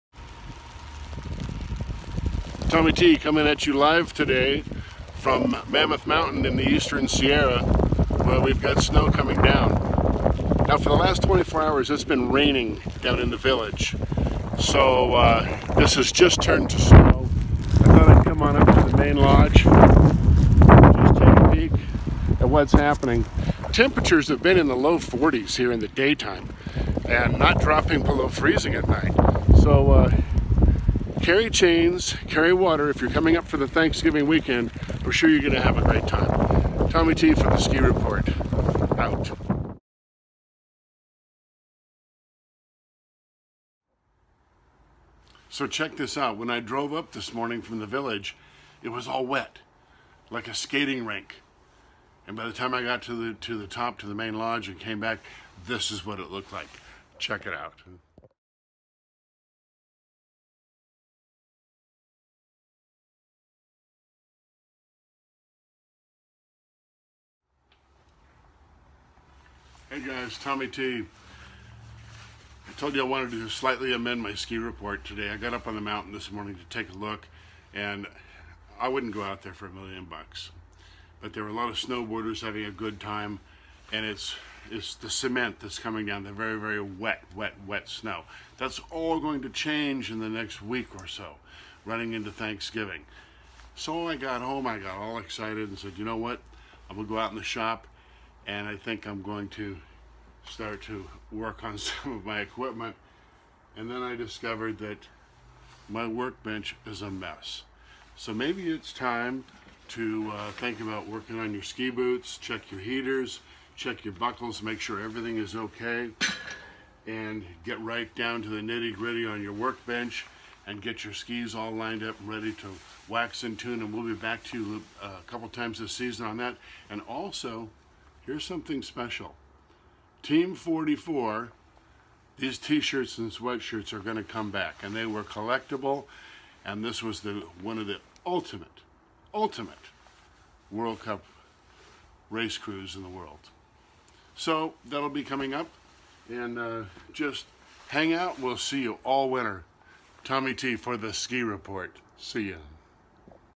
The ski report with Thomy t upcoming season 2018
Get excited start looking at your gear figuring out if you need some maintenance on your ski equipment and let’s have a great winter stay tuned for Ski tips with Thomy T